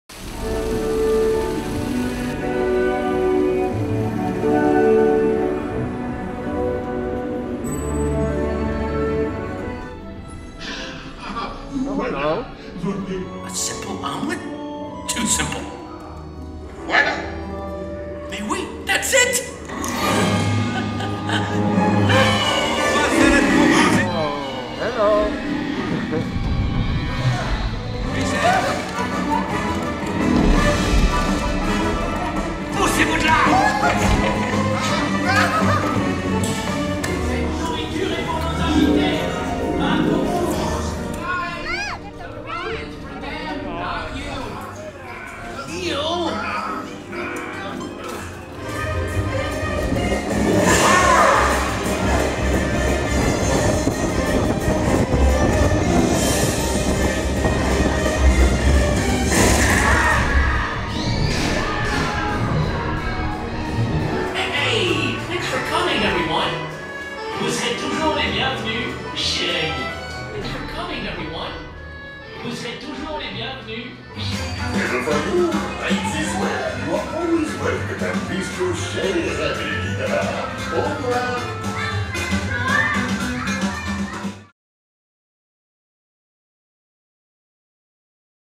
Remy's Ratatouille Adventure - Ride Cam
In what has to be one of the strangest Ride Cam experience's we've ever had (due to mask and 3d glasses), we give you Remy's Ratatouille Adventure!  Enjoy a sneak peek at the ride!